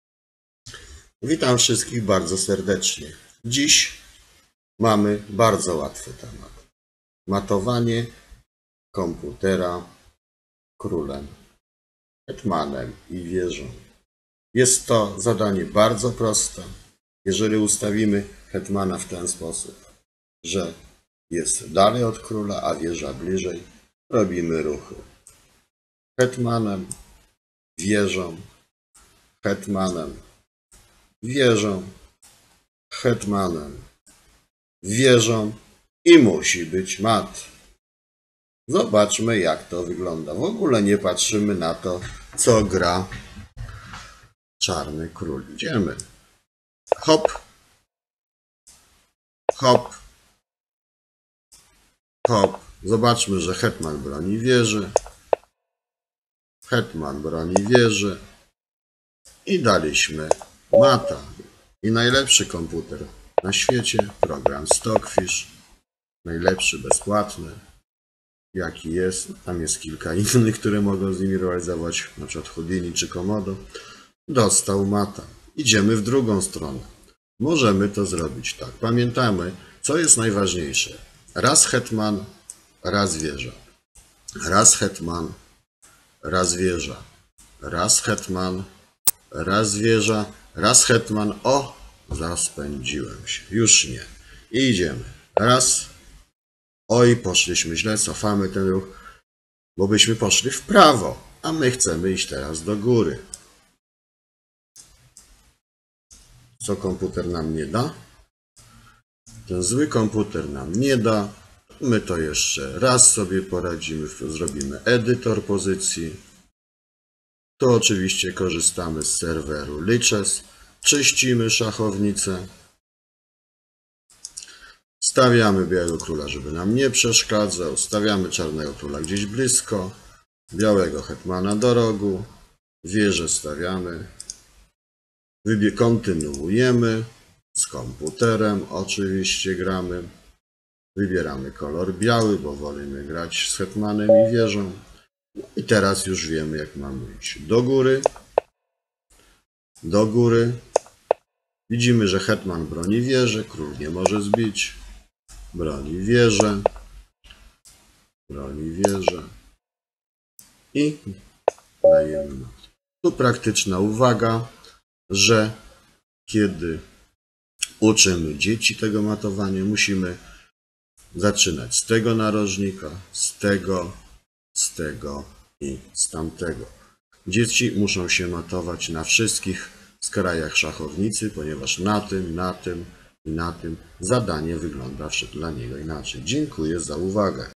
Szachy. Matujemy komputer Wieżą i Hetmanem.
Najbardziej prosty, czysto automatyczny sposób matowania przeciwnika. Silny program szachowy nie ma żadnych szans, żeby uniknąć słów "Szach i Mat!".